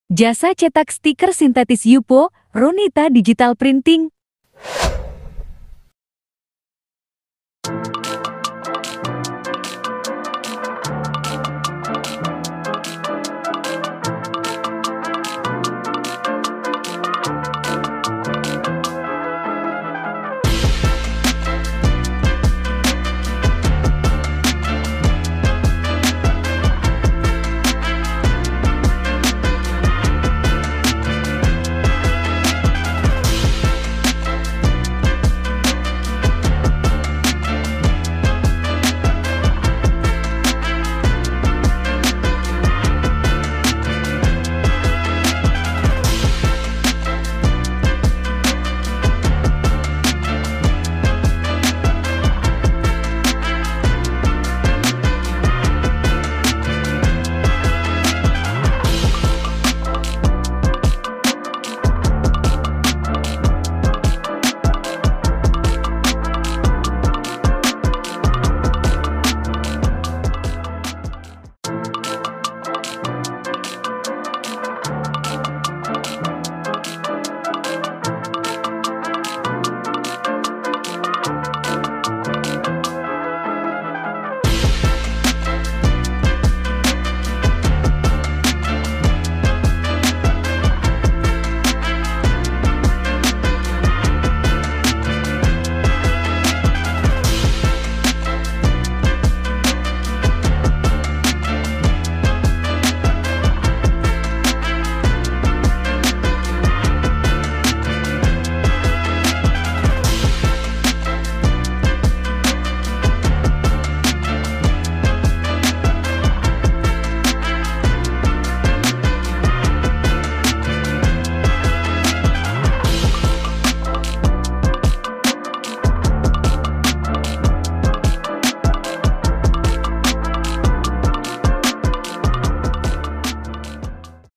Jasa Cetak Stiker Sintetis Yupo RONIta Digital Printing
–Harga 135.000/Meter
–Area cetak 90x90cm
–Dalam 1 lembar bisa di isi dengan beberapa desain

Stiker Yupo ini cocok banget buat label produl selain bahanya lebih tipis lemnya kuat dan bisa di tulis bagian yang tidak diprint

Yuk langsung di order aja 👇
WA:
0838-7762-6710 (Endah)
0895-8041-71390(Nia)